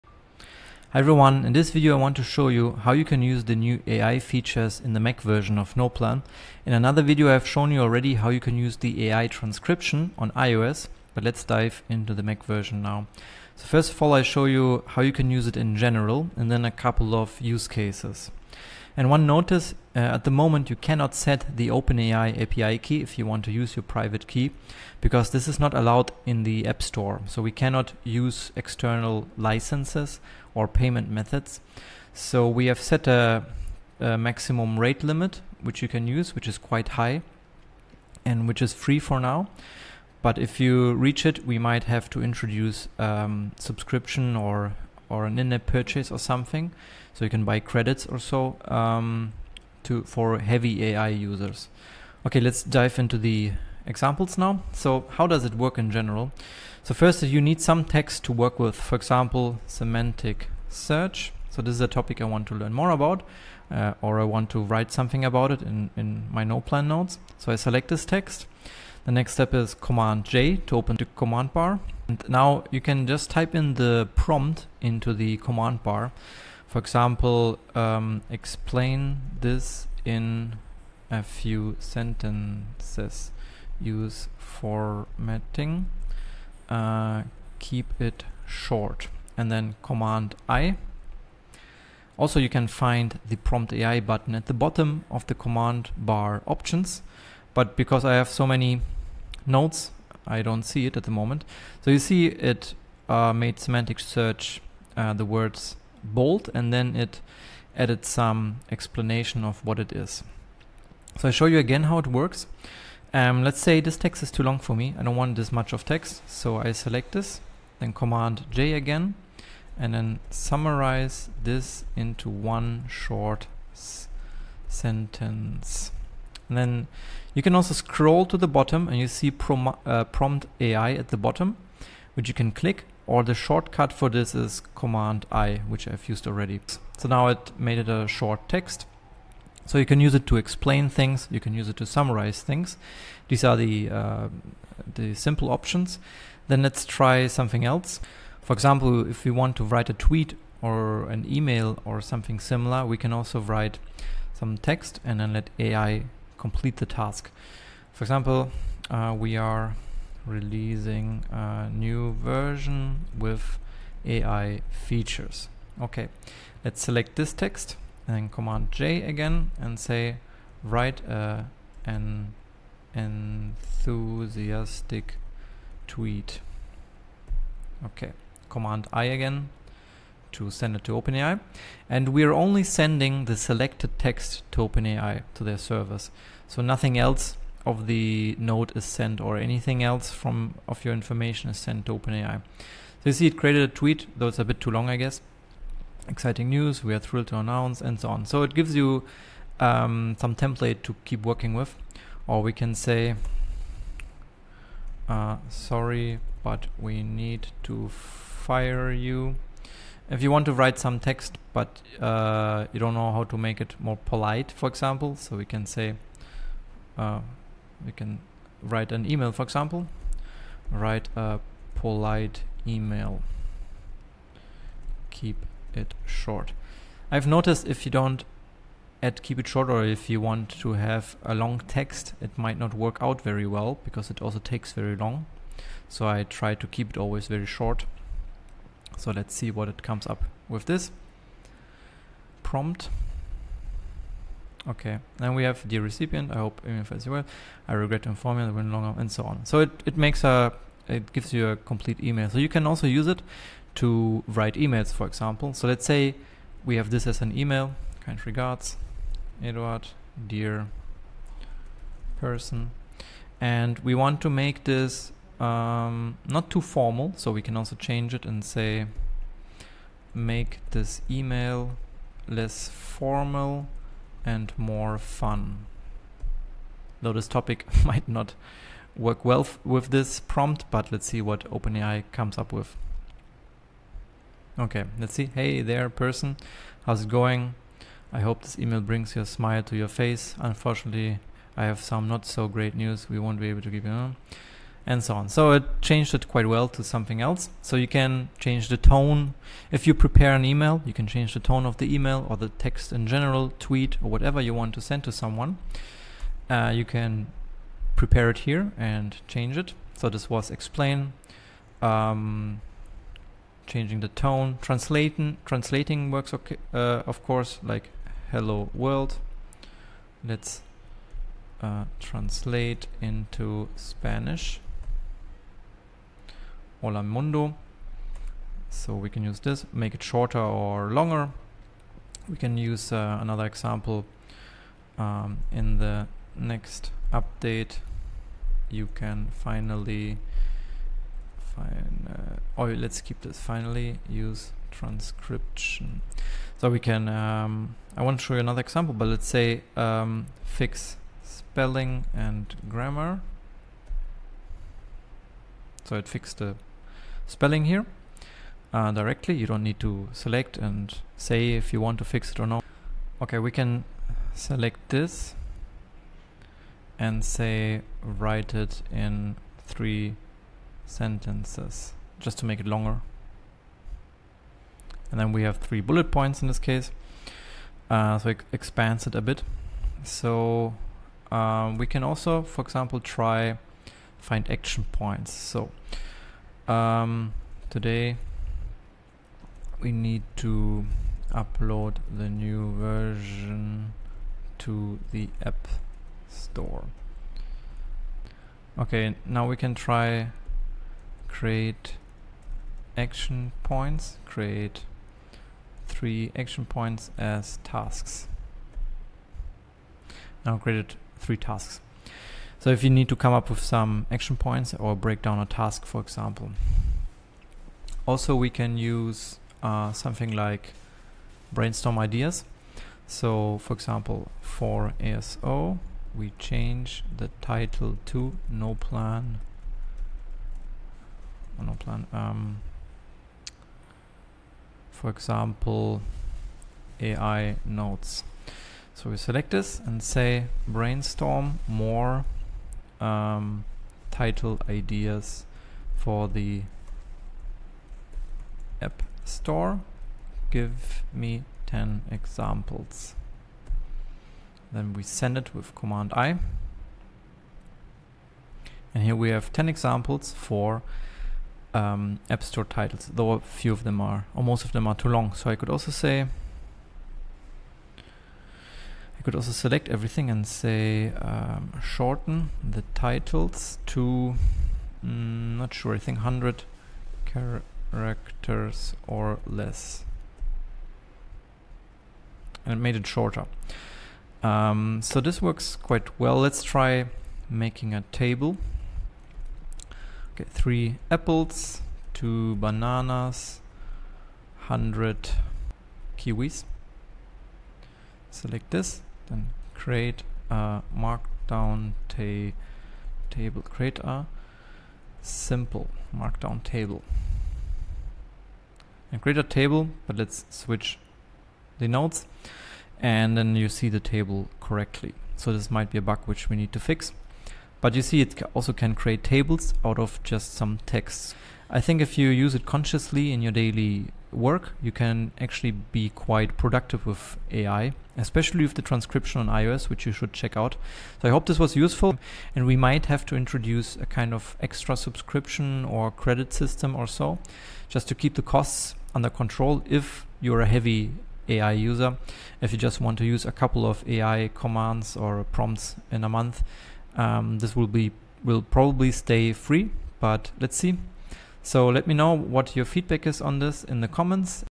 🤖 Discover the Power of AI (GPT-4) and make working with text more productive
Learn in this video how to get started with AI in NotePlan on Mac and make your days more productive working with text:

How to use it:
1. Select some text inside a note,
2. open the command bar using CMD+J,
3. type in your prompt (e.g., 'Explain this' or 'Summarize this'), and
4. press CMD+I

Examples:
- Text Summarization and Explanation: to get a simplified or detailed explanation use prompts like 'Explain this in 2 sentences' or 'Summarize this in a few bullet points'
- Content Creation: Whether you're drafting a tweet or an email, to generate templates or adjust the tone of your text.
- Other AI Features: Use AI for translation, fixing spelling and grammar mistakes, creating bulleted lists, identifying action points, brainstorming ideas, and creating tables from lists.